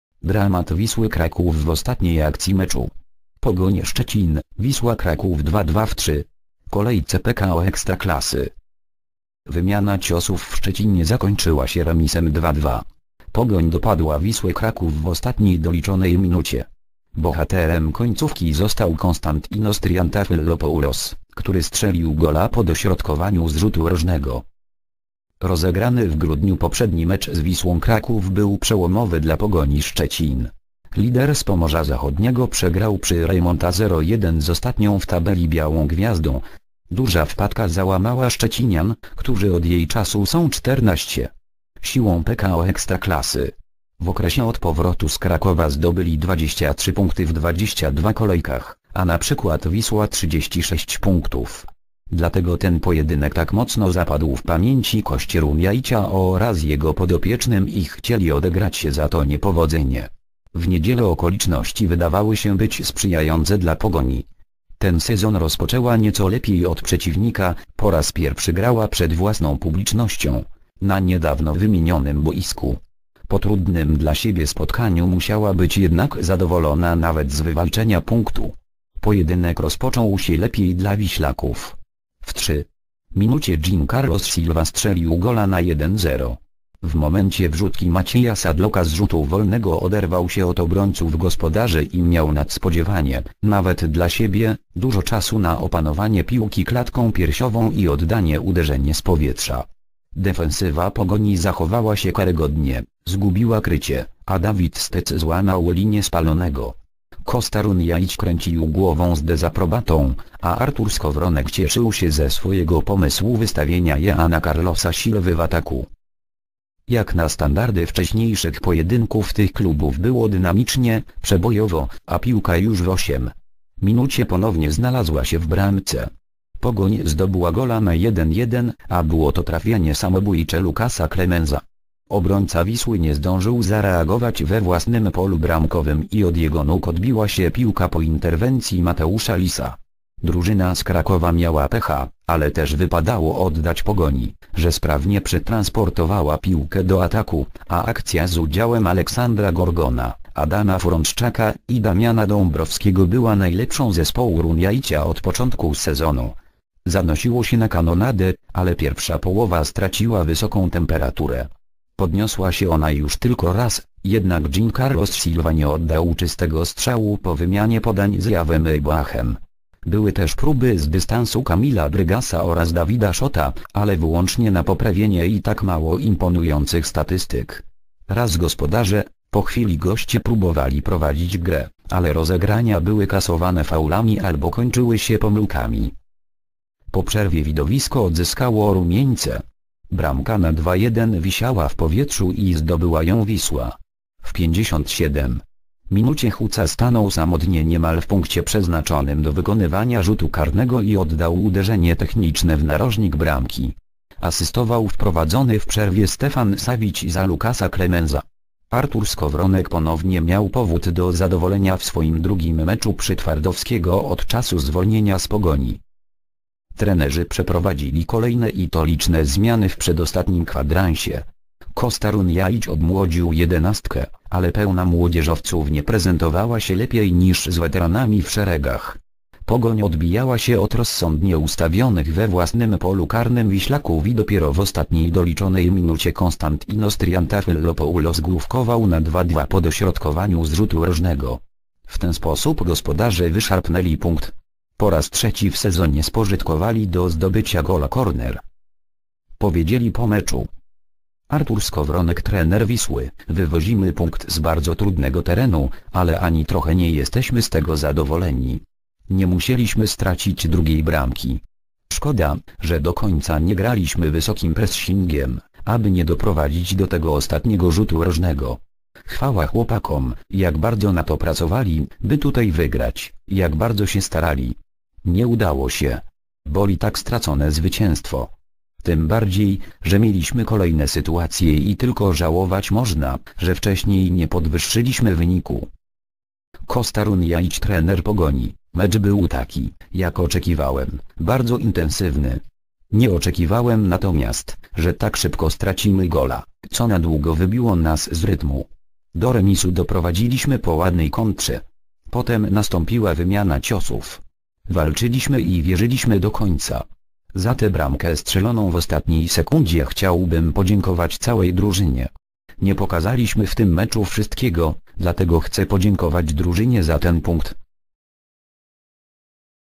Dramat Wisły Kraków w ostatniej akcji meczu!
Pogoń Szczecin - Wisła Kraków 2:2 w 3. kolejce PKO Ekstraklasy.

Pogoń Szczecin - Wisła Kraków 2:2 (1:1)

0:1 - Jean Carlos Silva 3'
1:1 - Lukas Klemenz (sam.) 8'
1:2 - Chuca 57'
2:2 - Konstantinos Triantafyllopoulos 90'

Żółte kartki: Smoliński, Gorgon (Pogoń) oraz Sadlok (Wisła)

Sędzia: Mariusz Złotek (Stalowa Wola)

Składy:

Pogoń: Dante Stipica - David Stec, Konstantinos Triantafyllopoulos, Benedikt Zech, Hubert Matynia - Damian Dąbrowski, Kamil Drygas (64' Maciej Żurawski), Kacper Smoliński (68' Santeri Hostikka) - Michał Kucharczyk (68' Sebastian Kowalczyk), Alexander Gorgon (80' Paweł Cibicki) - Adam Frączczak (64' Adrian Benedyczak)

Wisła: Mateusz Lis - Dawid Szot, Lukas Klemenz (46' Stefan Savić), Rafał Janicki, Maciej Sadlok -Vullnet Basha (84' Michał Mak), Gieorgij Żukow - Yaw Yeboah (65' David Niepsuj), Chuca (84' Rafał Boguski), Dawid Abramowicz - Jean Carlos Silva (65' Fatos Beciraj)
  (fot. PAP/Marcin Bielecki)